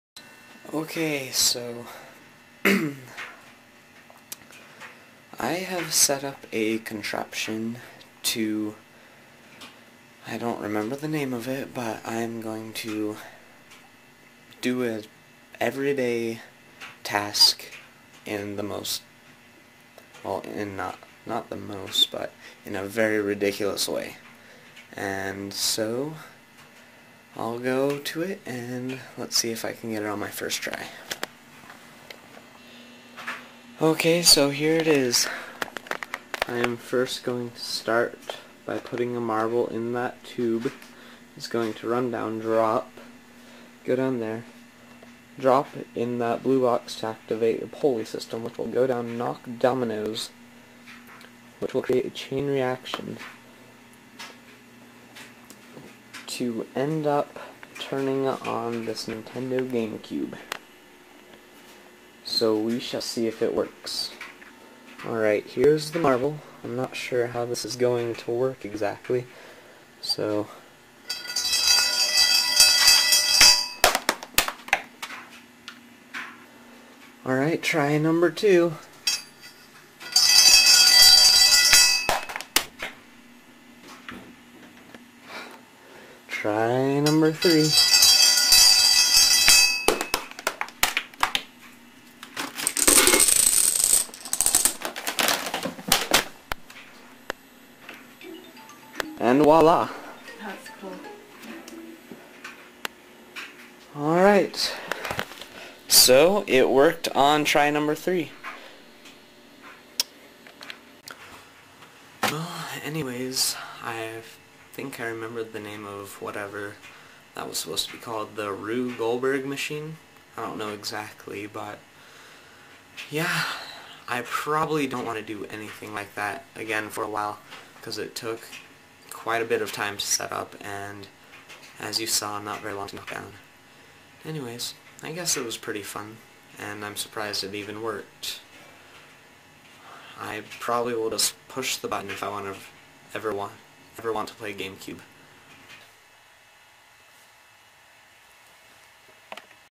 Rube Goldberg Machine
A delibratly over-engineered apparatus that preforms a very simple task in a very indirect and convoluted fashion usually using a chain reaction.  This is a Rube Goldberg Machine I made to turn on my gamecube.